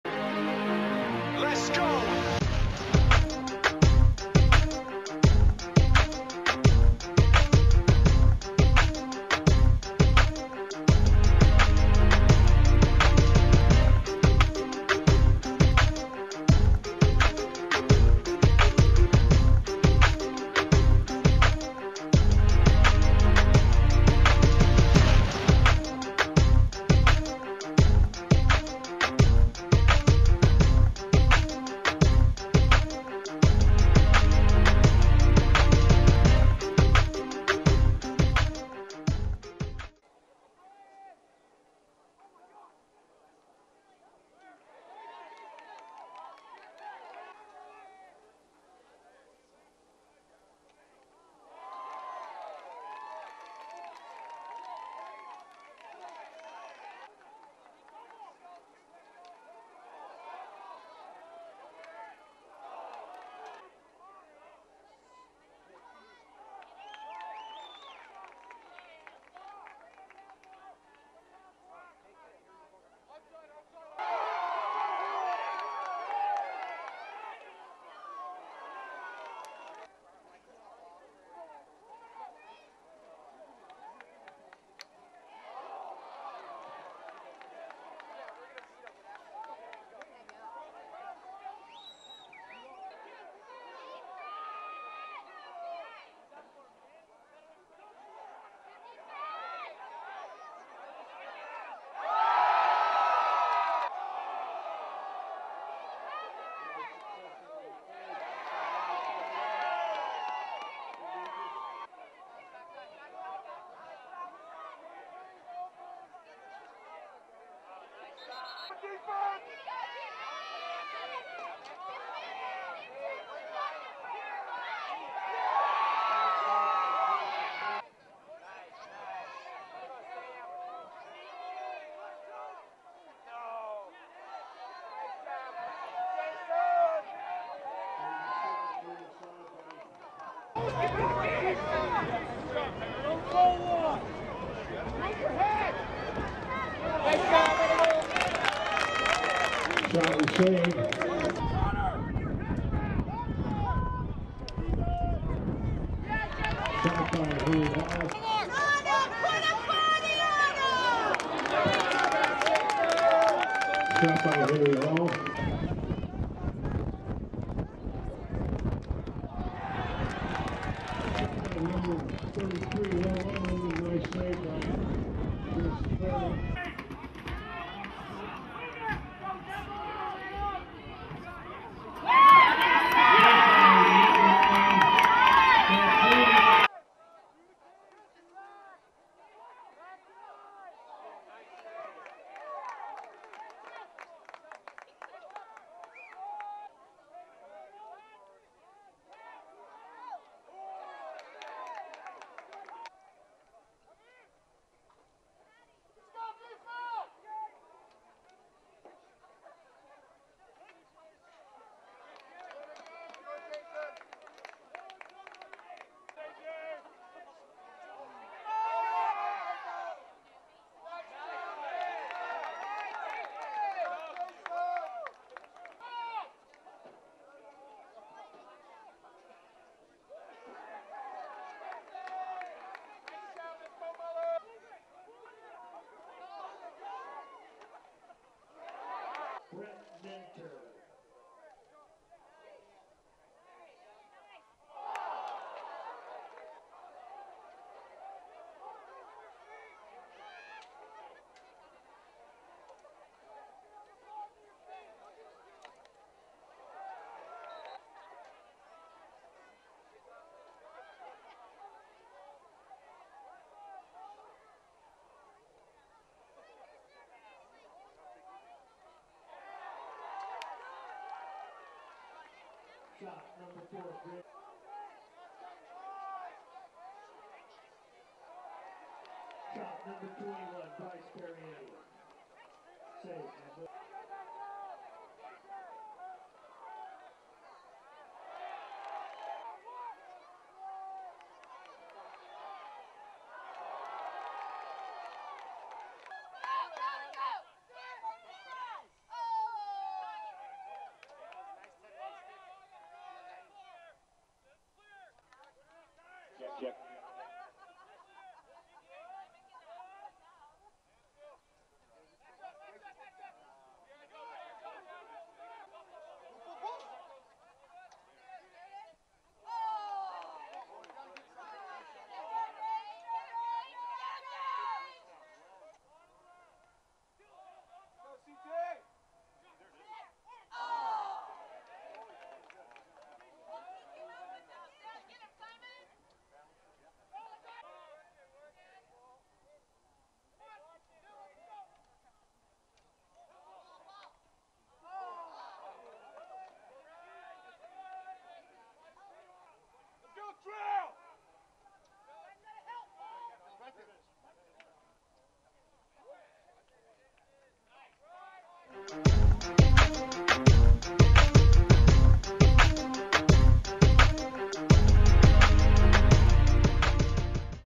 Jason Cleary: Lacrosse Video 2011
Jason's video includes a brief introduction followed by lacrosse highlights from the 2011 season.  Jason is a rising junior at JK Mullen High School in Denver, Colorado.